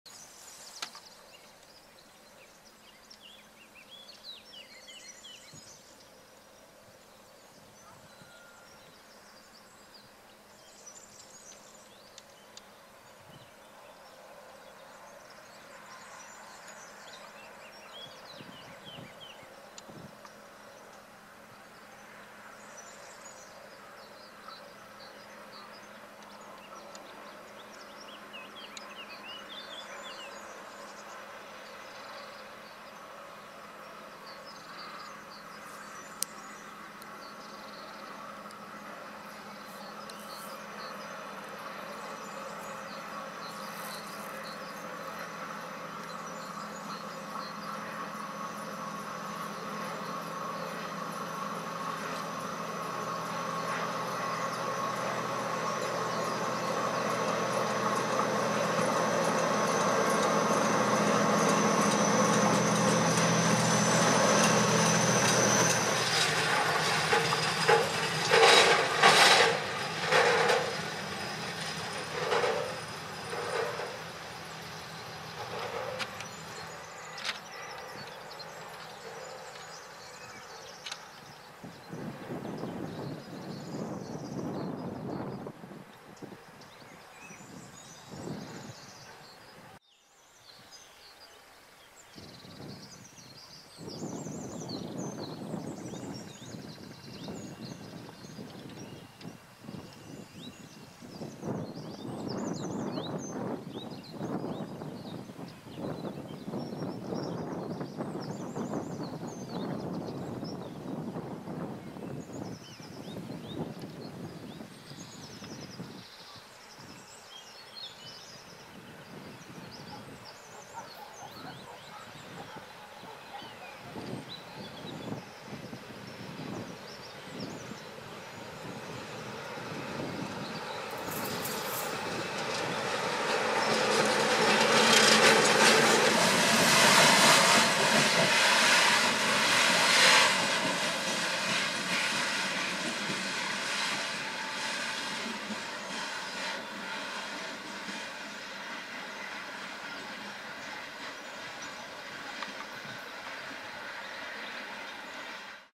Tren Arcelor abril 2013 / FZ200
Trenes siderúrgicos de ArcelorMittal circulando por la conexión interna que unen las factorías de Gijón y Avilés. Locomototas Vossloh G1700 BB y una B&W de 1970 aún en servicio.
Video tomado con cámara Panasonic FZ200 en AVCHD 50fps y editado con Magix.